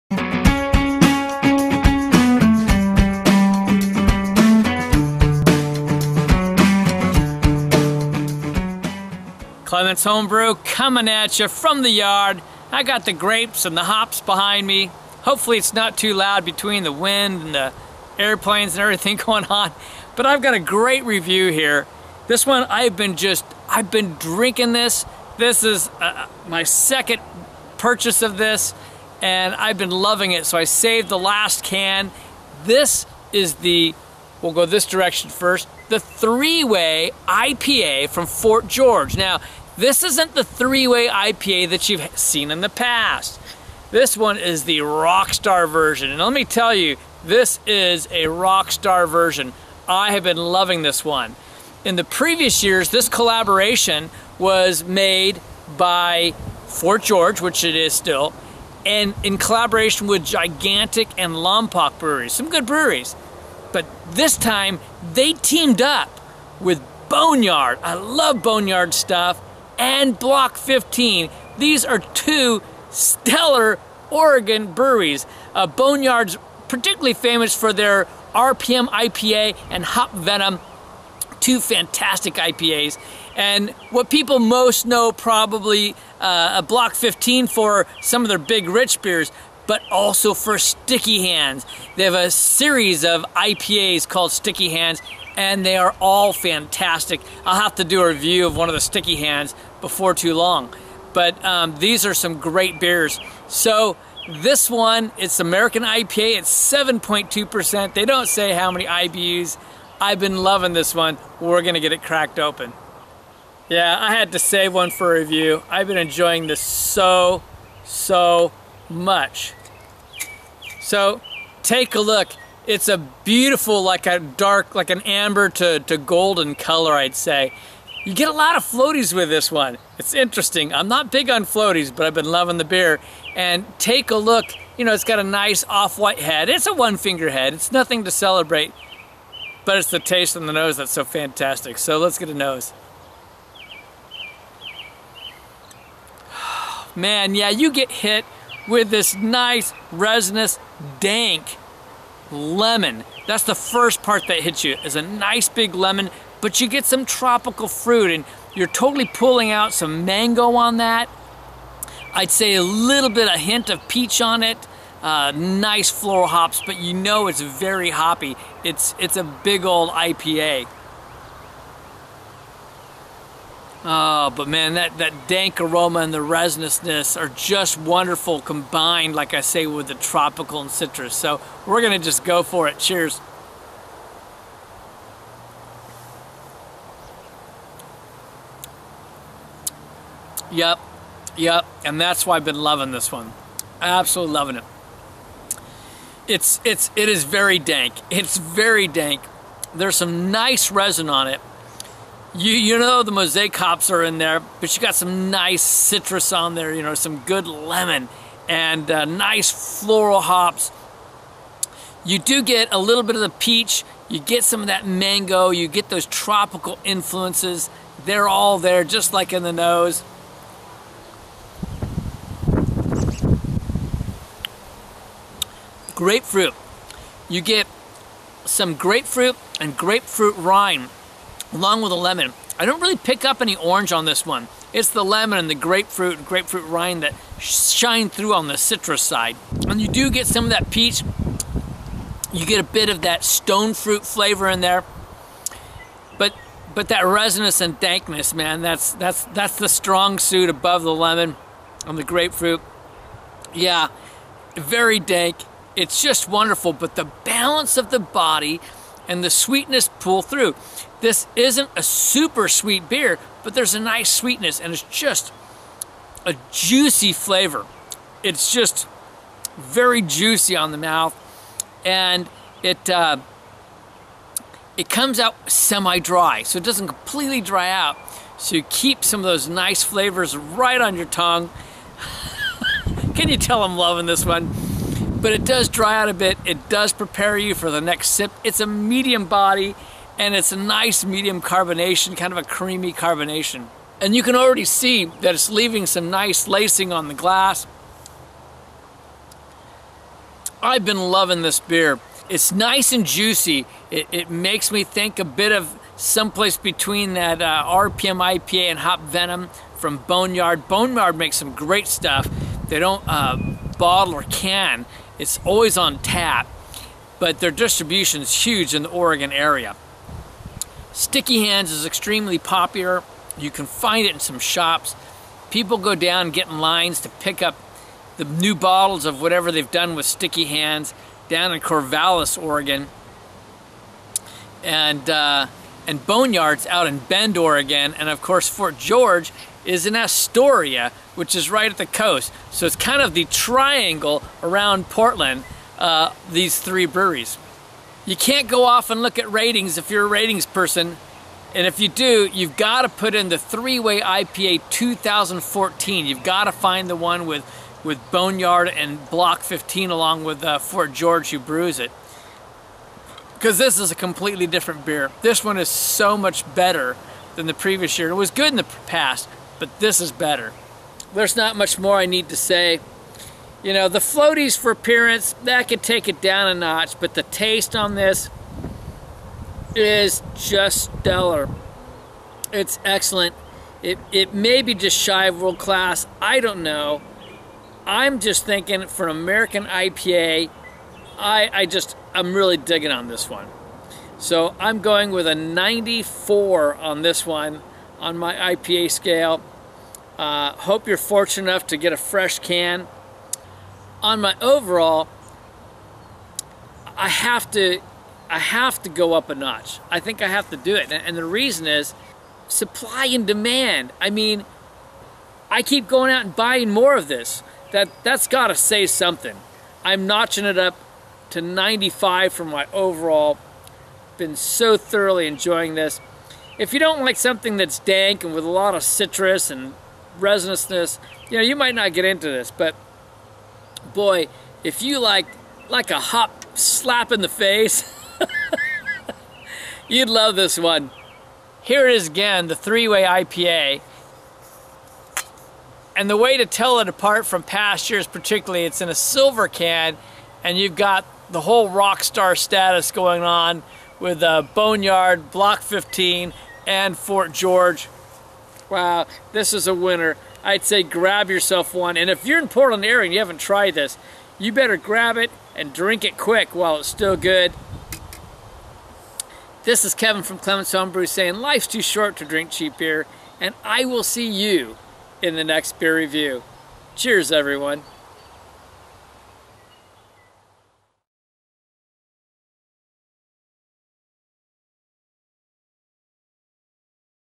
Beer Review 267: 3-Way IPA 2014 - Fort George / Block 15 / Boneyard
3-Way IPA 2014
Fort George Brewery - Astoria, Oregon
(Collaboration with Block 15 & Boneyard Breweries)

Style: American IPA
ABV: 7.2%